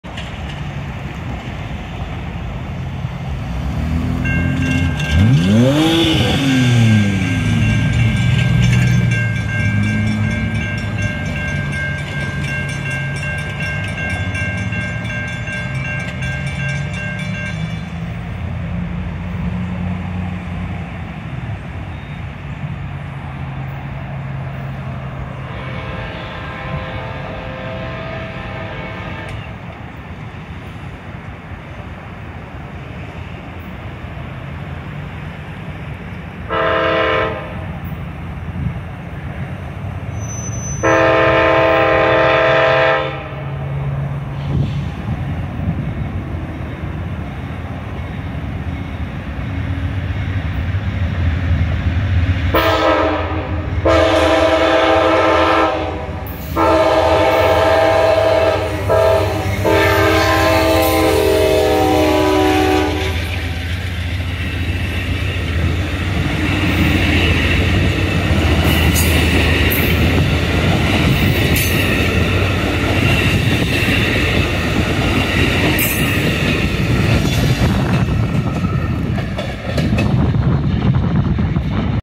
Amtrak City of New Orleans #58 (Northbound), departing New Orleans (NOL). 15-Mar-2022
Amtrak City of New Orleans #58 (Northbound), departing New Orleans (NOL). 15-Mar-2022. This location is Central Avenue, in Jefferson, Louisiana. The train is about fifteen minutes out of the station.
AMTK 83, a GE P42DC, pulls 7 Superliners: 2 Sleepers, 1 Diner, 1 Cafe/Lounge, 3 Coaches.